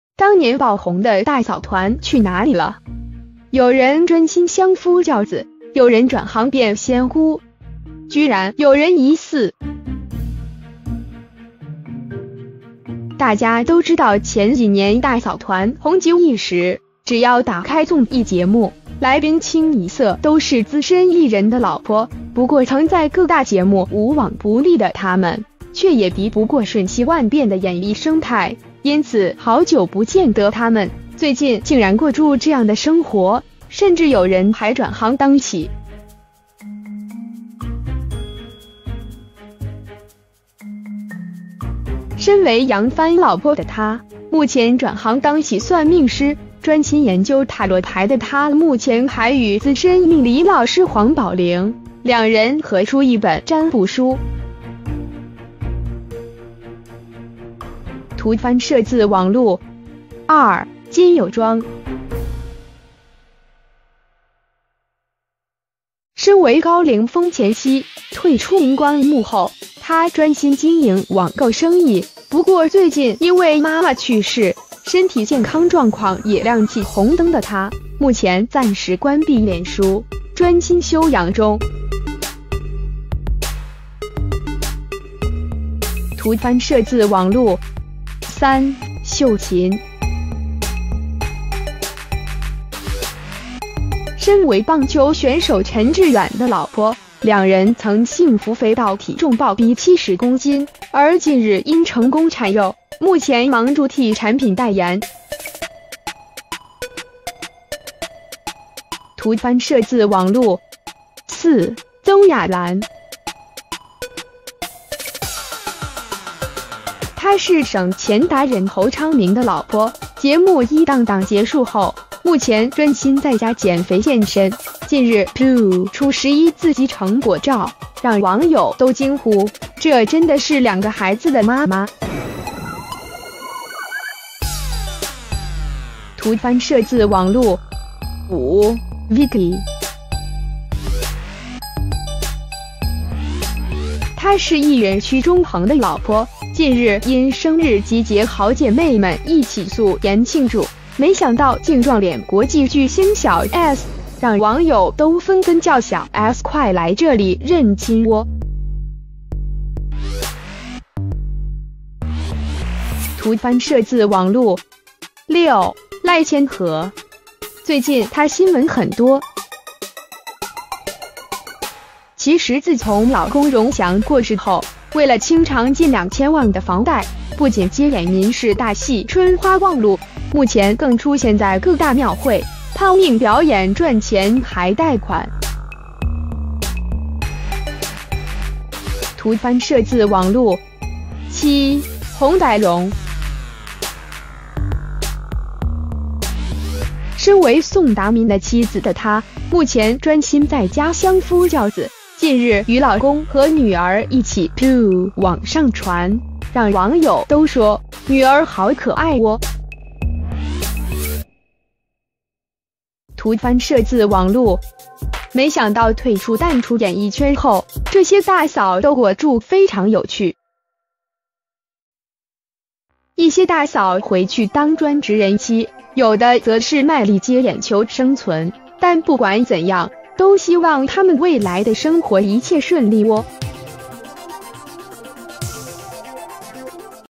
當年爆紅的「大嫂團」去哪裡了？有人專心相夫教子、有人轉行變「仙姑」！居然有人疑似…..
大家都知道前幾年「大嫂團」紅極一時，隻要打開縱一節目，來賓清一色都是資深藝人的老婆，不過曾在各大節目無往不利的她們，卻也敵不過瞬息萬變的演藝生態，因此好久不見得她們，最近竟然過著這樣的生活，甚至有人還轉行當起…
身為揚帆老婆的她，目前轉行當起算命師，專心研究塔羅牌的她目前還與資深命理老師黃寶靈，兩人合出一本佔卜書！

圖/翻攝自網路

二、金友莊

身為高淩風前妻，退出螢光幕後，她專心經營網購生意，不過最近因為媽媽去逝，身體健康狀況也亮起紅燈的她，目前暫時關閉臉書，專心休養中。
圖/翻攝自網路

三、秀琴

身為棒球選手陳致遠的老婆，兩人曾幸福肥到體重爆逼70公斤，而近日因成功鏟肉，目前忙著替產品代言。
圖/翻攝自網路

四、曾雅蘭

她是省錢達人侯昌明的老婆，節目一檔檔結束後，目前專心在家減肥健身，近日po出11字肌成果照，讓網友都驚呼：「這真的是兩個孩子的媽嗎？」
圖/翻攝自網路

五、Vicky

她是藝人屈中恆的老婆，近日因生日集結好姐妹們一起素顏慶祝，沒想到竟撞臉國際巨星小S，讓網友都紛紛叫小S快來這裡認親喔！
圖/翻攝自網路